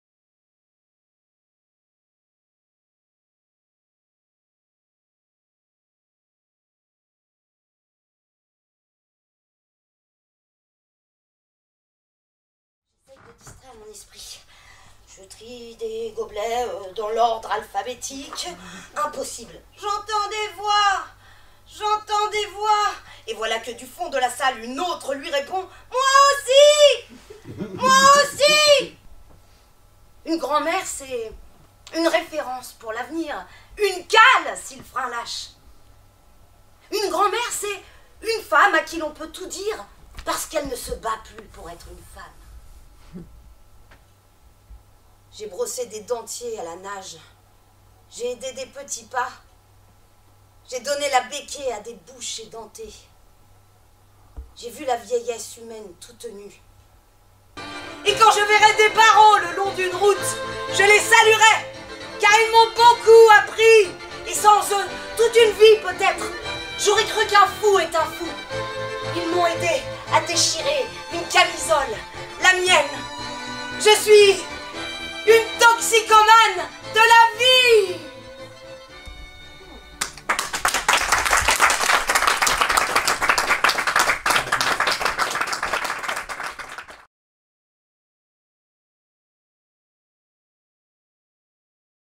Le journal FOU d'une infirmière
Compagnie au jour d'aujourd'hui, d'après le roman d' Anne-Xavier Albertini.De et avec Prune Lichtlé, mise en scène Thierry Jahn, Lumière André Diot.Une pièce de théâtre cet été en programmation du féstival d'Avignon.